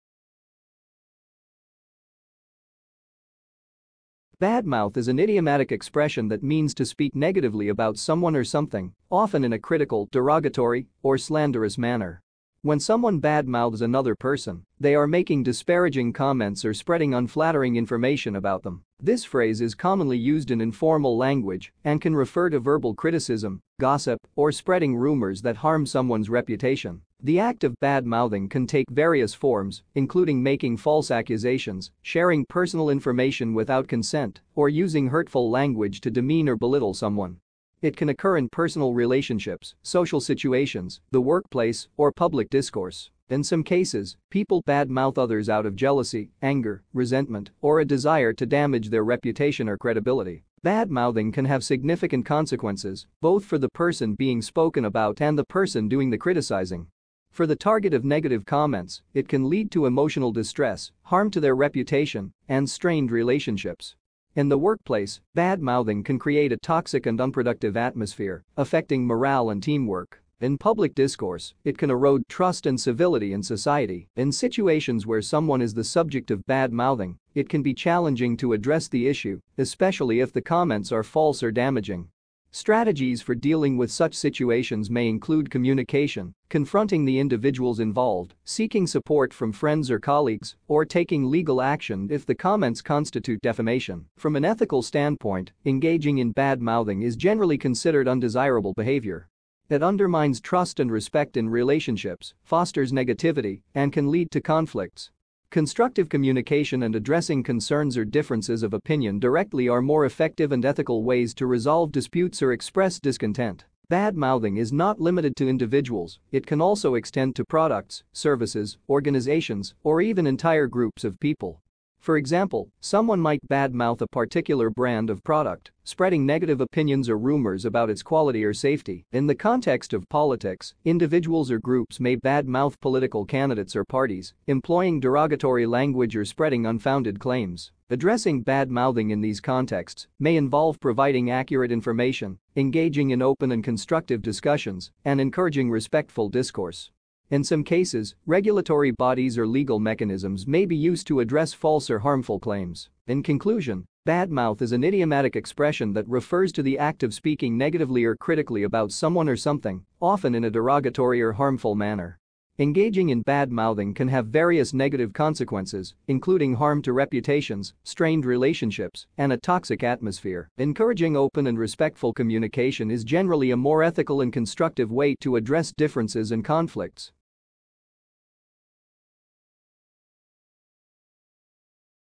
American Expression E1936 Bad mouth
"Bad mouth" is an idiomatic expression that means to speak negatively about someone or something, often in a critical, derogatory, or slanderous manner. When someone "bad mouths" another person, they are making disparaging comments or spreading unflattering information about them. This phrase is commonly used in informal language and can refer to verbal criticism, gossip, or spreading rumors that harm someone's reputation.

The act of "bad mouthing" can take various forms, including making false accusations, sharing personal information without consent, or using hurtful language to demean or belittle someone. It can occur in personal relationships, social situations, the workplace, or public discourse. In some cases, people "bad mouth" others out of jealousy, anger, resentment, or a desire to damage their reputation or credibility.

"Bad mouthing" can have significant consequences, both for the person being spoken about and the person doing the criticizing. For the target of negative comments, it can lead to emotional distress, harm to their reputation, and strained relationships. In the workplace, bad mouthing can create a toxic and unproductive atmosphere, affecting morale and teamwork. In public discourse, it can erode trust and civility in society. In situations where someone is the subject of "bad mouthing," it can be challenging to address the issue, especially if the comments are false or damaging. Strategies for dealing with such situations may include communication, confronting the individuals involved, seeking support from friends or colleagues, or taking legal action if the comments constitute defamation.

From an ethical standpoint, engaging in "bad mouthing" is generally considered undesirable behavior. It undermines trust and respect in relationships, fosters negativity, and can lead to conflicts. Constructive communication and addressing concerns or differences of opinion directly are more effective and ethical ways to resolve disputes or express discontent.

"Bad mouthing" is not limited to individuals; it can also extend to products, services, organizations, or even entire groups of people. For example, someone might "bad mouth" a particular brand of product, spreading negative opinions or rumors about its quality or safety. In the context of politics, individuals or groups may "bad mouth" political candidates or parties, employing derogatory language or spreading unfounded claims. Addressing "bad mouthing" in these contexts may involve providing accurate information, engaging in open and constructive discussions, and encouraging respectful discourse. In some cases, regulatory bodies or legal mechanisms may be used to address false or harmful claims.

In conclusion, "bad mouth" is an idiomatic expression that refers to the act of speaking negatively or critically about someone or something, often in a derogatory or harmful manner. Engaging in "bad mouthing" can have various negative consequences, including harm to reputations, strained relationships, and a toxic atmosphere. Encouraging open and respectful communication is generally a more ethical and constructive way to address differences and conflicts.